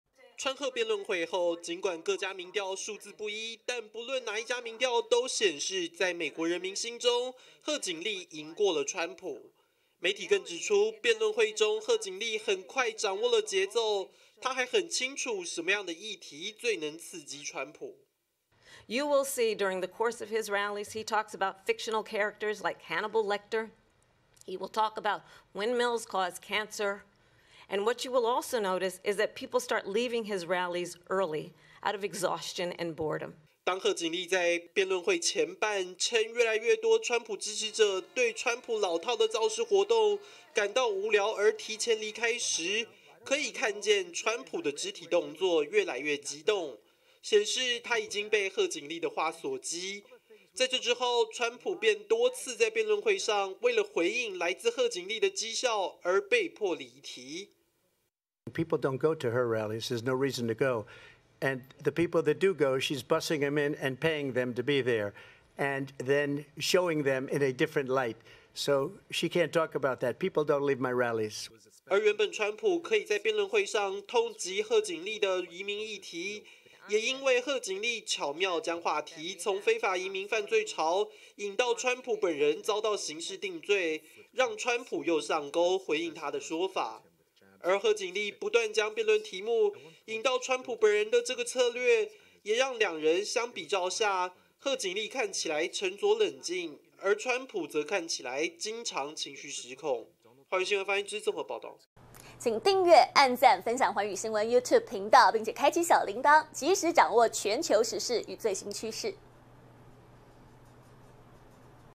川普.賀錦麗辯論戰誰贏了？美媒最新民調曝光｜寰宇新聞
雖然美國總統候選人辯論會結束後，兩方都各自宣告勝利。但外國媒體普遍認為，川普這次表現較差，主要原因是賀錦麗，成功的在辯論會上戳到川普痛處，導致他被激怒而打亂節奏。